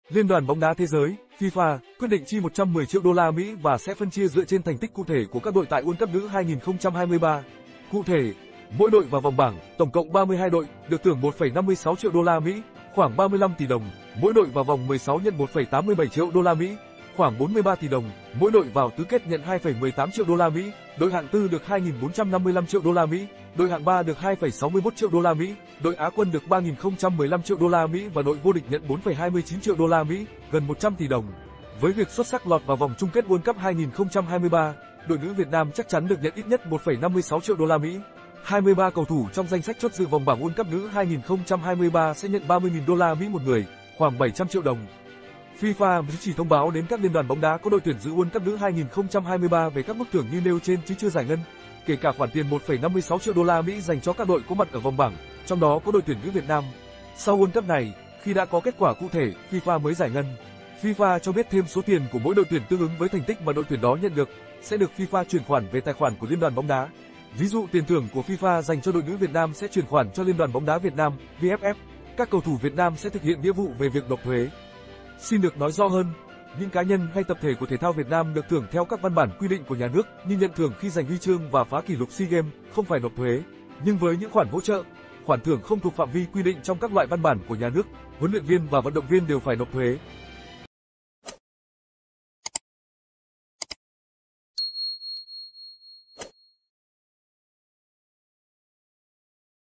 ĐỘI TUYỂN NỮ VIỆT NAM NỘP THUẾ THU NHẬP BẤT THƯỜNG TỪ KHOẢN THƯỞNG CỦA FIFA
Liên đoàn Bóng đá thế giới (FIFA) quyết định chi 110 triệu USD và sẽ phân chia dựa trên thành tích cụ thể của các đội tại World Cup nữ 2023. Cụ thể, mỗi đội vào vòng bảng (tổng cộng 32 đội) được thưởng 1,56 triệu USD (khoảng 35 tỉ đồng); mỗi đội vào vòng 16 nhận 1,87 triệu USD (khoảng 43 tỉ đồng); mỗi đội vào tứ kết nhận 2,18 triệu USD; đội hạng tư được 2,455 triệu USD; đội hạng ba được 2,61 triệu USD; đội á quân được 3,015 triệu USD và đội vô địch nhận 4,29 triệu USD (gần 100 tỉ đồng). Với việc xuất sắc lọt vào vòng chung kết World Cup 2023, đội nữ Việt Nam chắc chắn được nhận ít nhất 1,56 triệu USD. 23 cầu thủ trong danh sách chốt dự vòng bảng World Cup nữ 2023 sẽ nhận 30.000 USD/người (khoảng 700 triệu đồng).